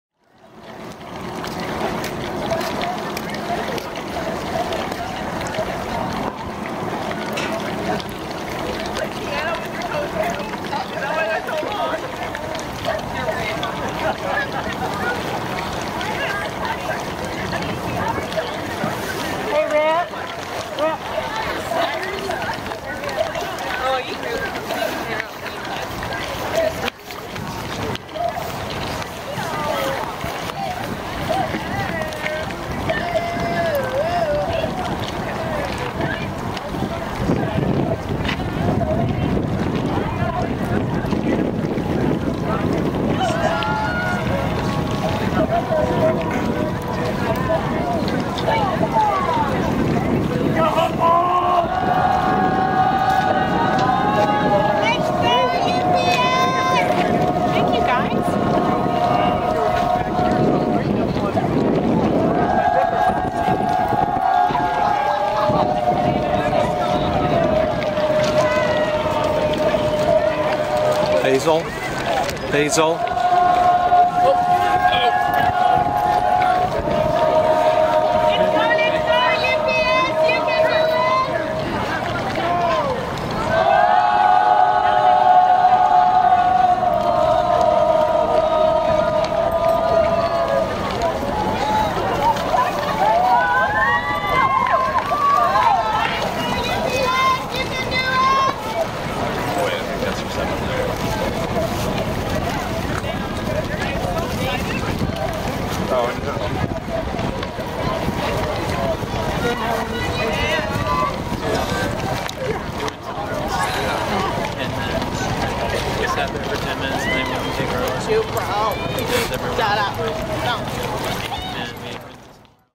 HSU Novice 8 @ 2014 NCRC Championships
This video is about Humboldt State University Women's Crew Novice 8+ boat at the 2014 NCRC Championships at Dexter Lake, Oregon.